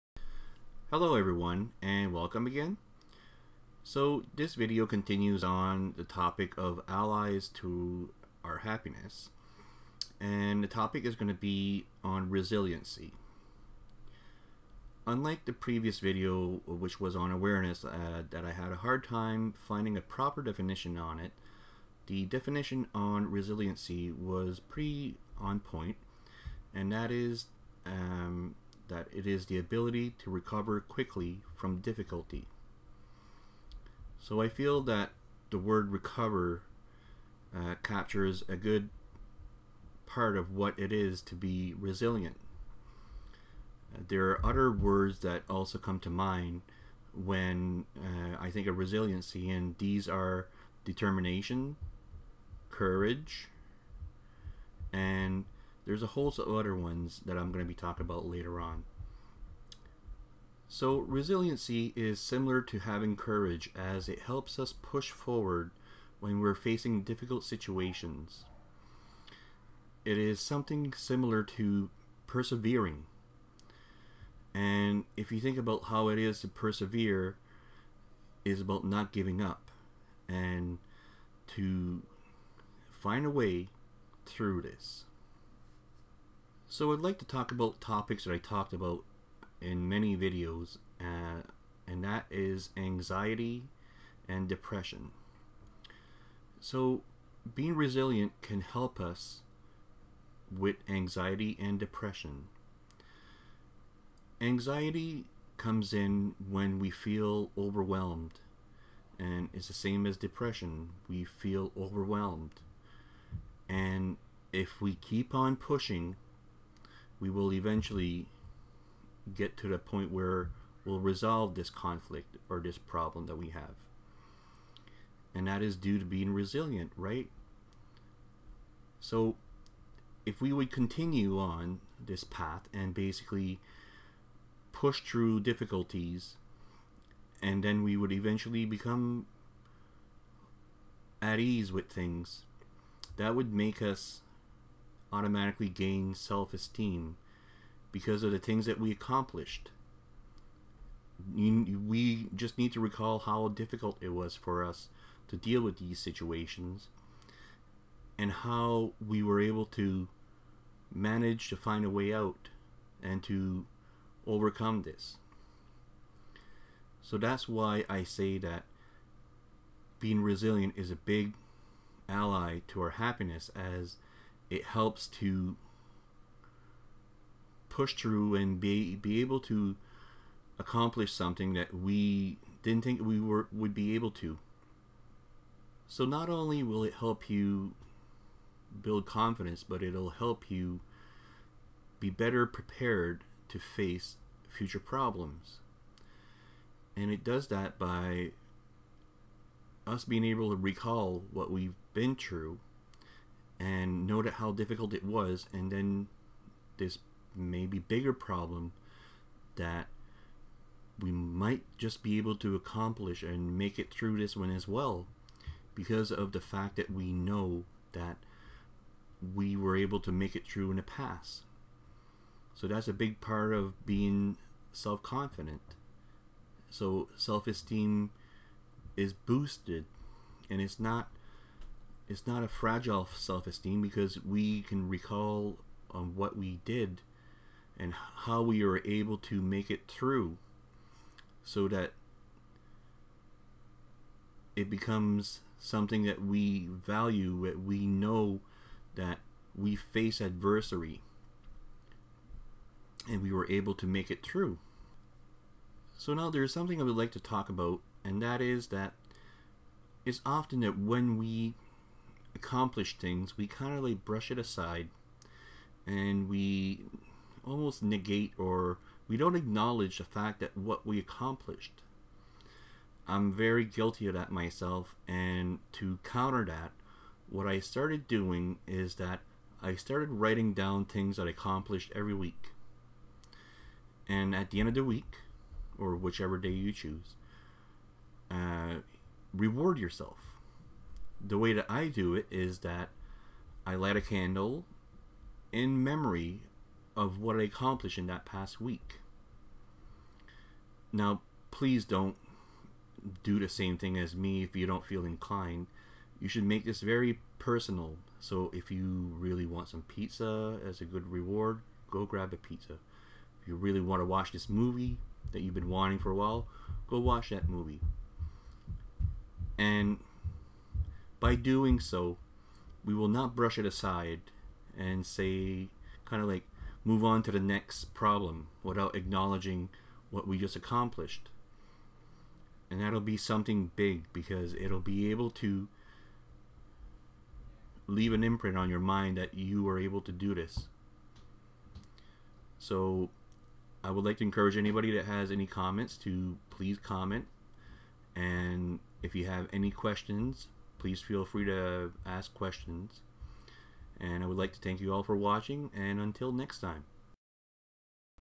Resiliency: ally to happiness
In this video, I talk about resiliency as an ally to happiness.  Resiliency is similar to courage, determination and perserverance as it helps push through difficulty and not to give up.  If applied, resiliency can help with 3 problems I often talk about, and that is anxiety, depression and low self-esteem.